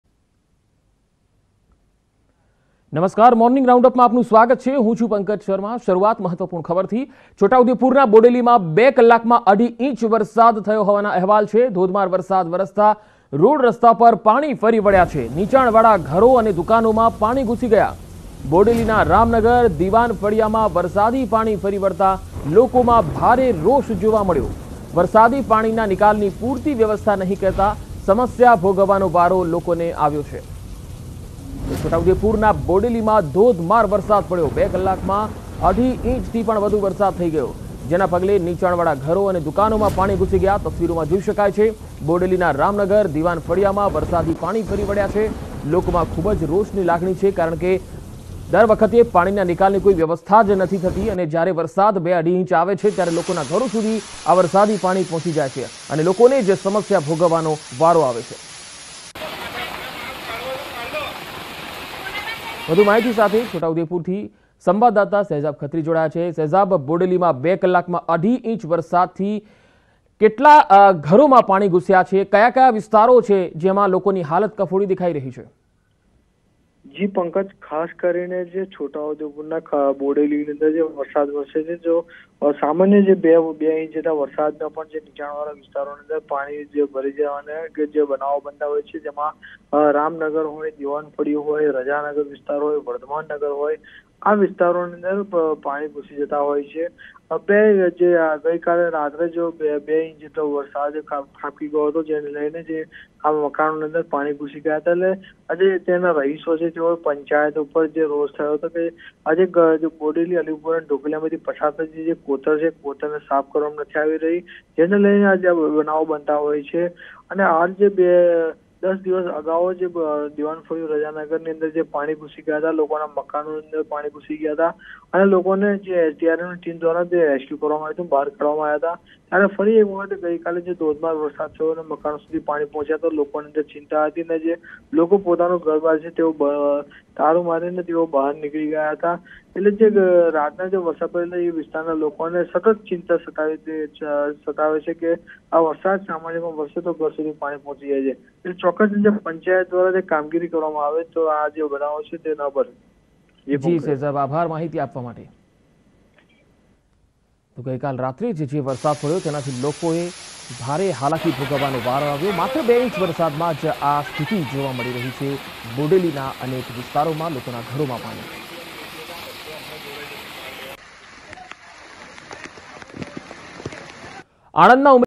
Gujarat Rain:છોટા ઉદેપુરના બોડેલીમાં 2 કલાકમાં અઢી ઈંચ વરસાદ | News18 Gujarati
Two and a half inches of rain fell in Bodley in 2 hours. Water entered people's houses. People expressed their anger as the system did not provide proper arrangements.

બોડેલીમાં 2 કલાકમાં અઢી ઈંચ વરસાદ ખાબક્યો. લોકોના ઘરોમાં ઘુસ્યા પાણી. તંત્ર દ્વારા યોગ્ય વ્યવસ્થા ન કરાતા લોકોએ રોષ વ્યક્ત કર્યો.